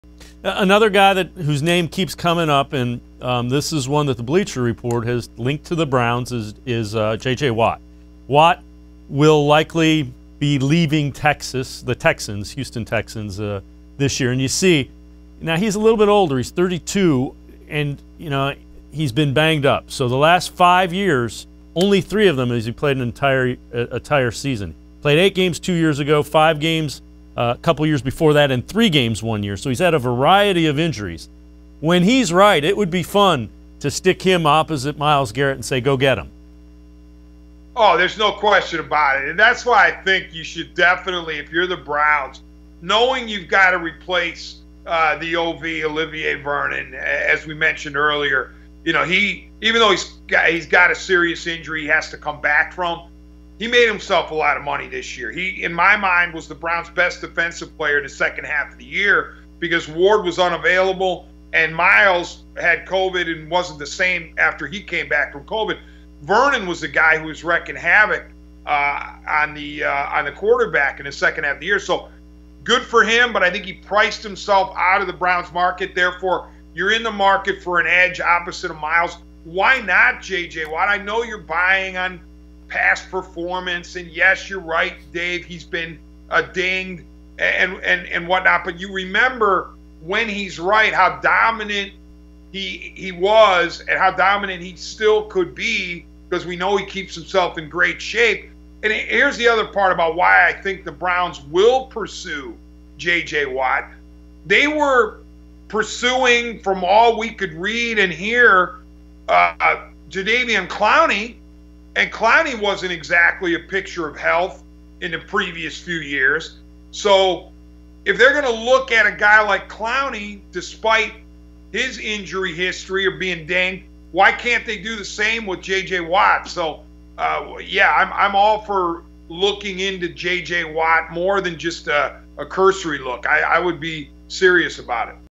JJ Watt linked to Browns -MS&LL 2-3-21
Several sources are reporting the Browns have expressed an interest in JJ Watt.